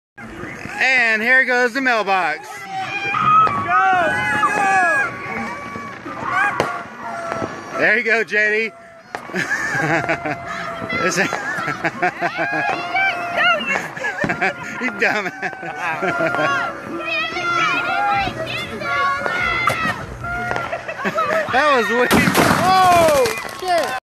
Texas mailbox explosion
How to blow up a mailbox in Texas.  This was my buddies mailbox and we had permission and it was replaced.  Nobody was hurt during this.